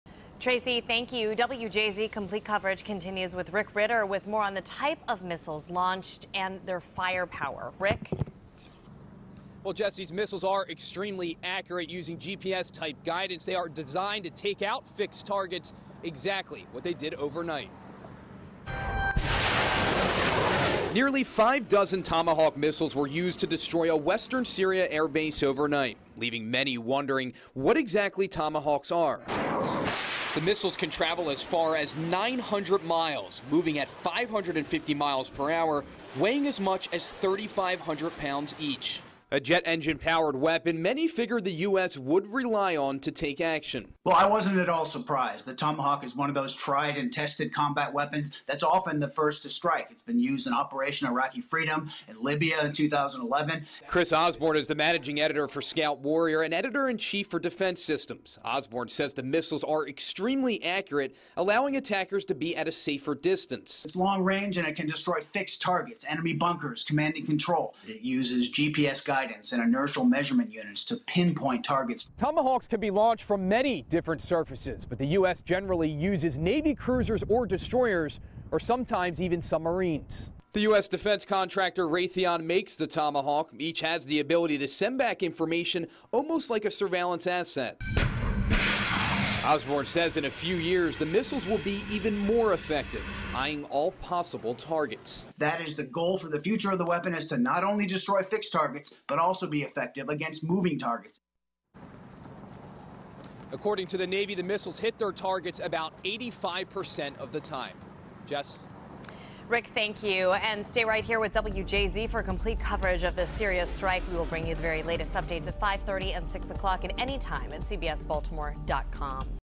What Exactly is A Tomahawk Missile?
Rick Ritter reports.